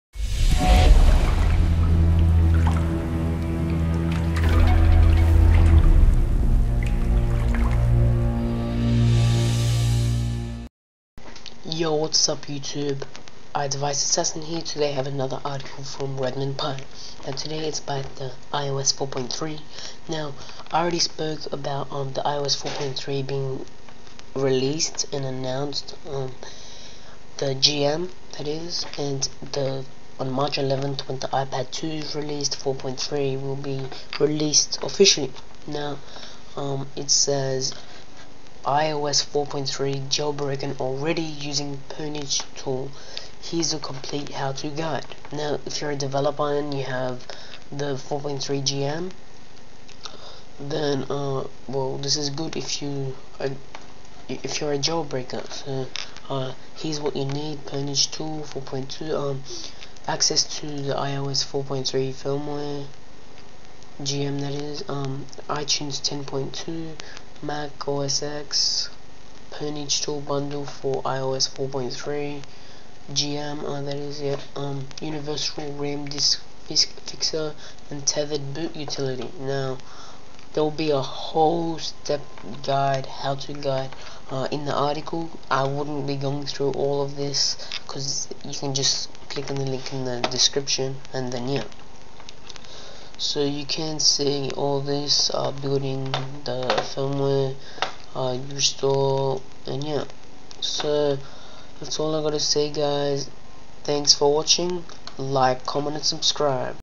How to Jailbreak NEW iOS 4.3 GM with PwnageTool
This is just a video about the article I read on Redmond Pie about the iOS 4.3 Gold Master Firmware which is now able to be Jailbroken with PwnageTool which means that you will need a Mac

Game Center: iDeviceAssassin